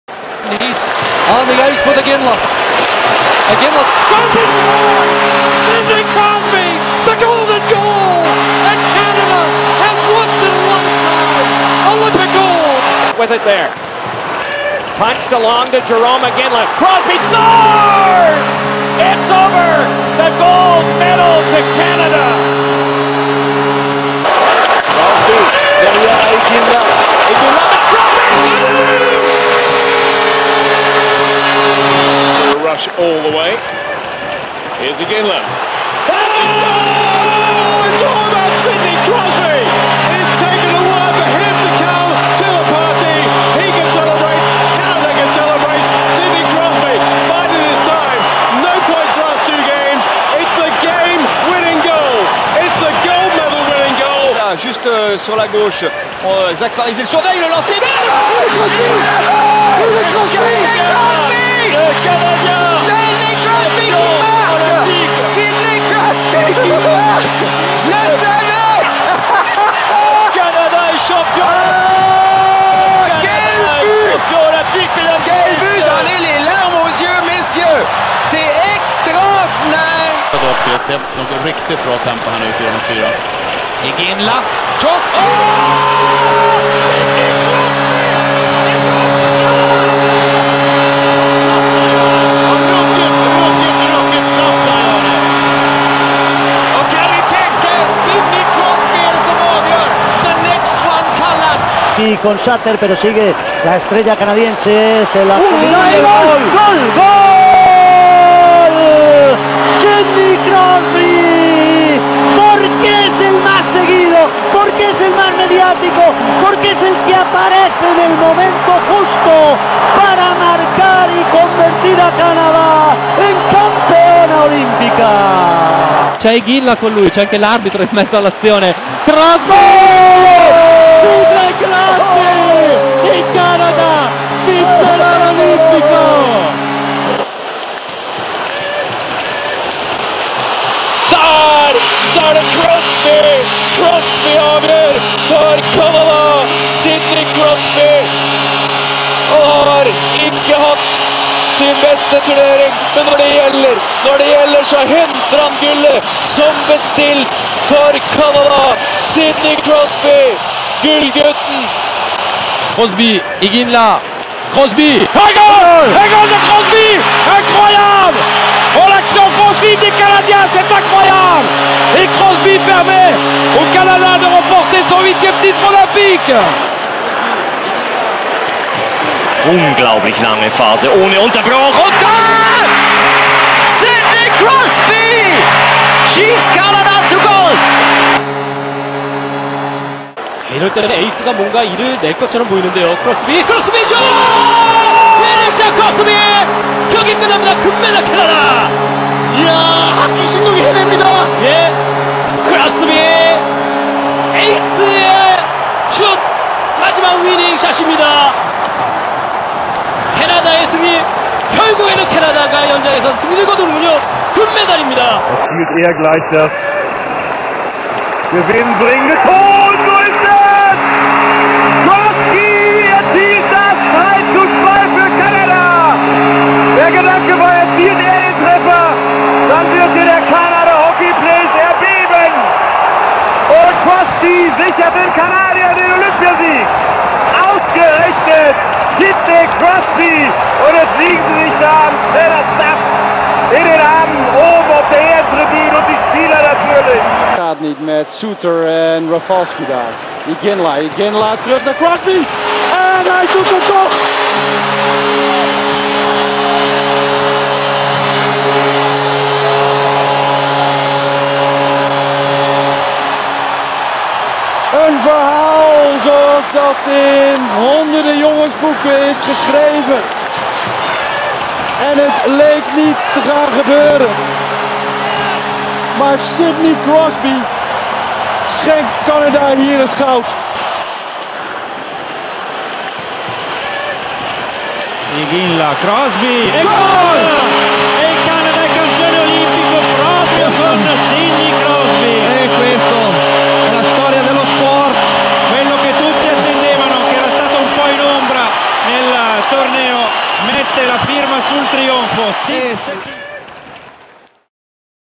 Sidney Crosby Golden Goal - Heard from 16 different TV Broadcasts - Olympics 2010 (HD)
Sidney Crosby's Overtime goal vs Team USA in 16 different TV-Stations around the globe at the Olympics 2010.

Big thanks to "plenarius" for bringing all the audio clips.